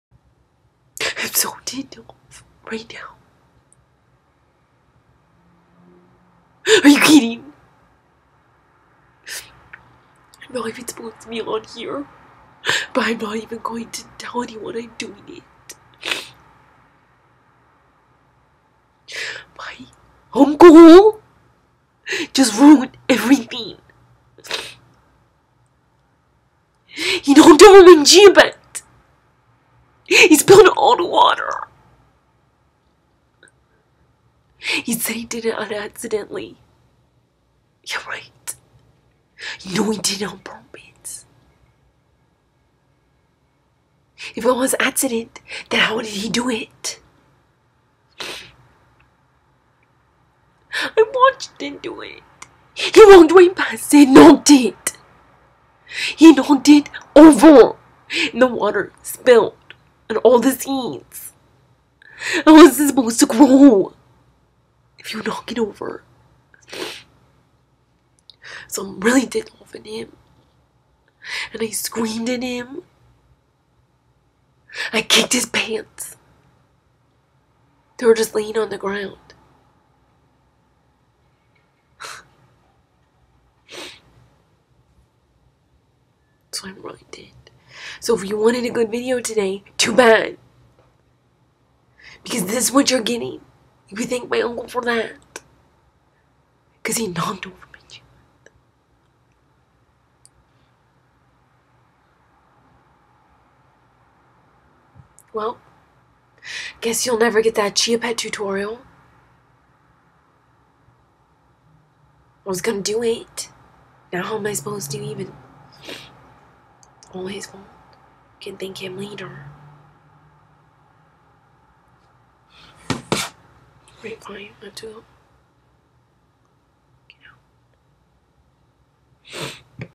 ME CRYING BECAUSE OF MY UNCLE
Im so mad at him right now i cant believ he would do this to me. dont even try to devend him or else.

TICKET INFO FOR MY TOUR!
(Go here for all concert ticket info)